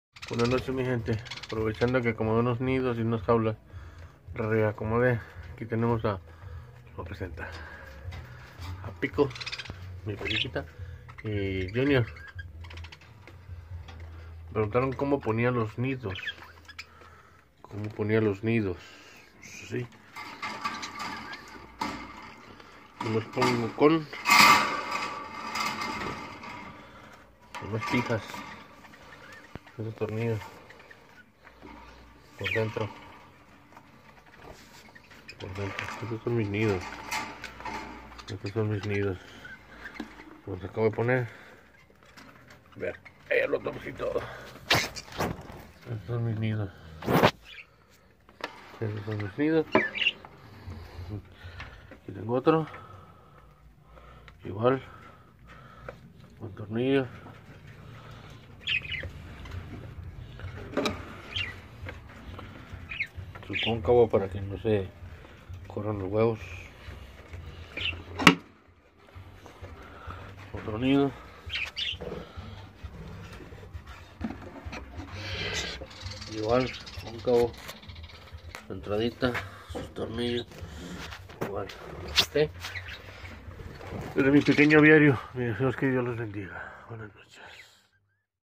Como fijo mis nidos en la JAULA..
Arrancamos Nueva TEMPORADA..2023..
y Si Gustan Medidas delos Nidos..
Con gusto Selas Enviamos..
( El codigo QR en el video
..ES MI AVIARIO EN EL FACE.)